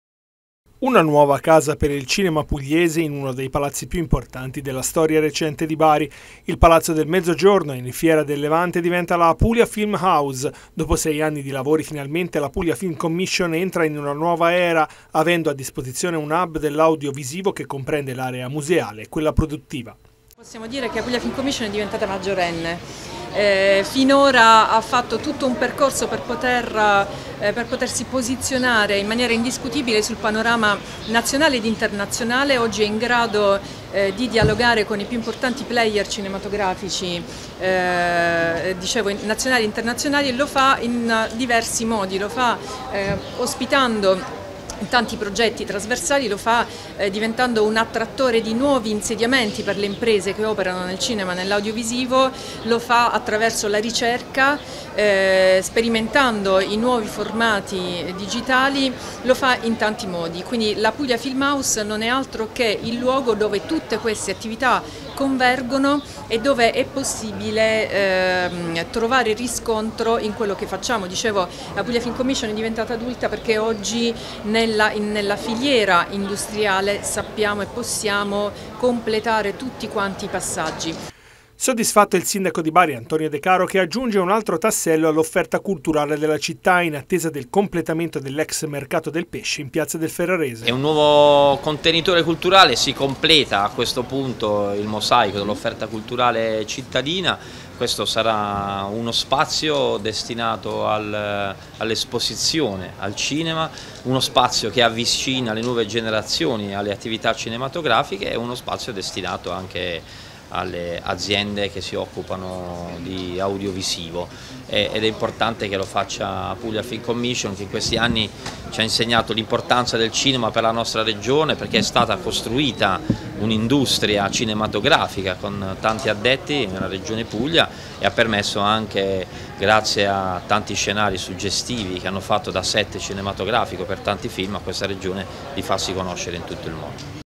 Bari, nasce l'Apulia Film House: "Una casa per il cinema pugliese"
Lo storico Palazzo del Mezzogiorno in Fiera del Levante entra in una nuova era